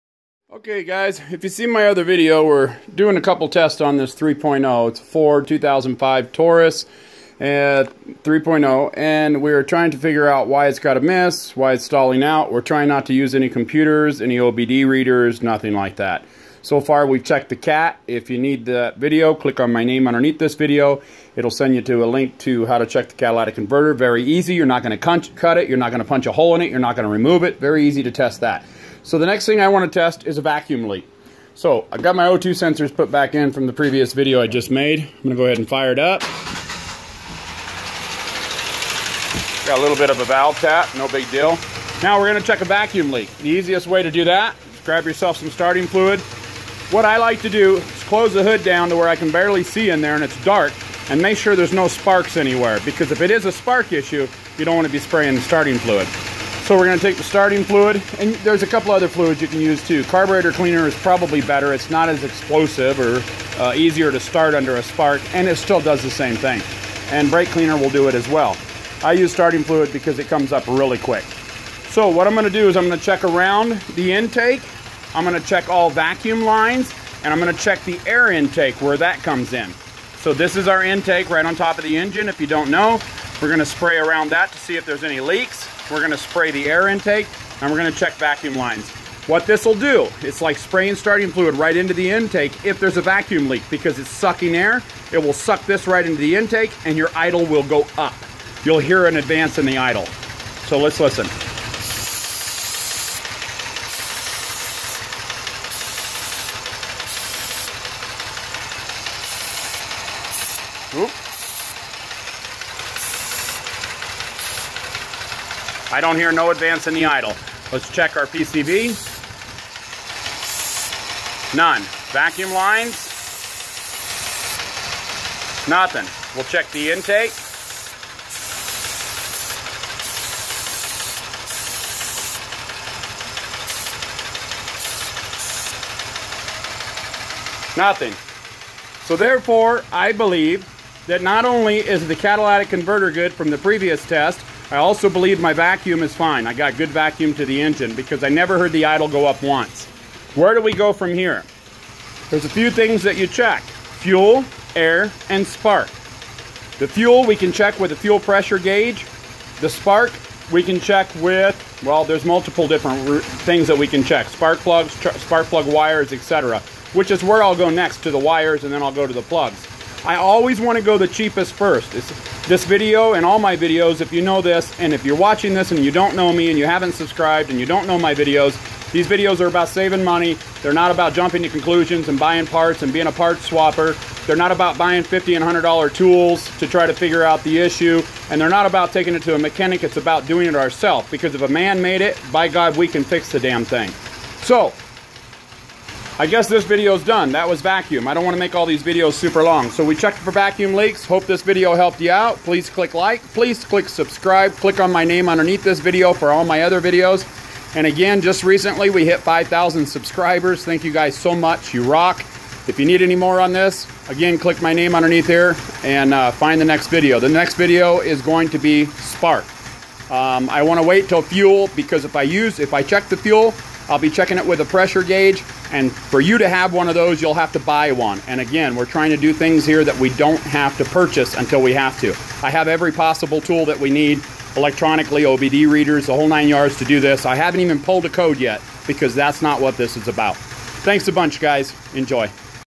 Vacuum Leak Test.
How to check where a vacuum leak is coming from easily. Enjoy!!!